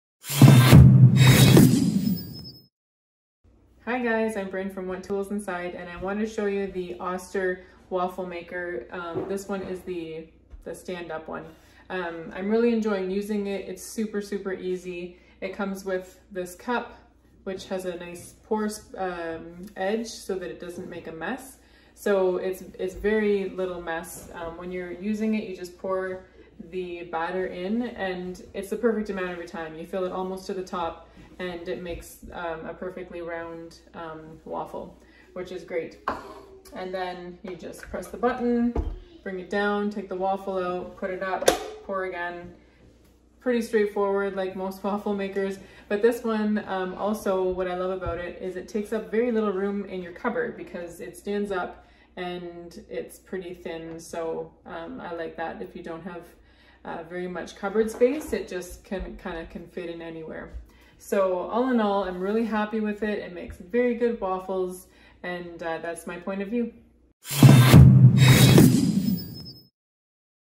Our Point of View on Oster Waffle Makers
Check Our New Website For Amazing Deals!
(Commissionable Links)

about this item:
Measures 10. 72" L x 4. 97" W x 11. 77" H, Cord measures 27" L, Easy-open, button Safe-to-touch handle prevents burns, PFOA- and PTFE-free, Ceramic/metal/plastic, Wipe clean. 1080 watts
Includes a measuring cup to prevent holes and undercooked spots
DuraCeramic nonstick cookware is easy to clean and won't flake or peel
Unique vertical orientation and wide top spout make for easy, mess-free pouring
Prepare a perfect Belgian-style waffle in minutes